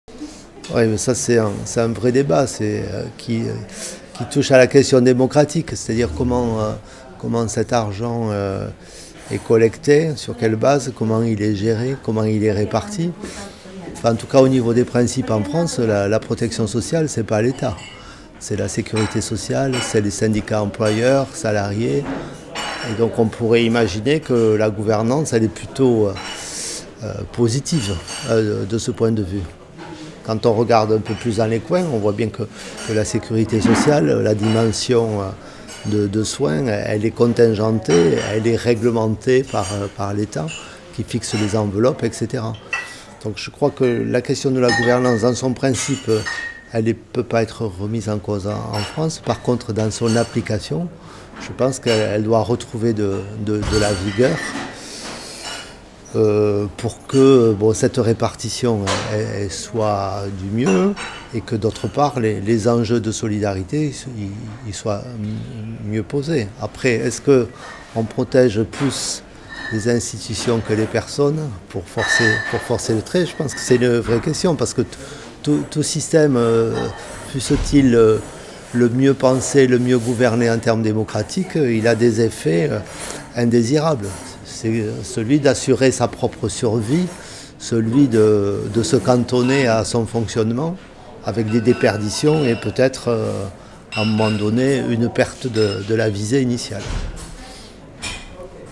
LES ORGANISATIONS D'ACCOMPAGNEMENT DES PERSONNES VULNÉRABLES DOIVENT ELLES REFON
Serge Loubet, administrateur de l'Udaf 11, France, répond à la question: LES ORGANISATIONS D'ACCOMPAGNEMENT DES PERSONNES VULNÉRABLES DOIVENT ELLES REFON...?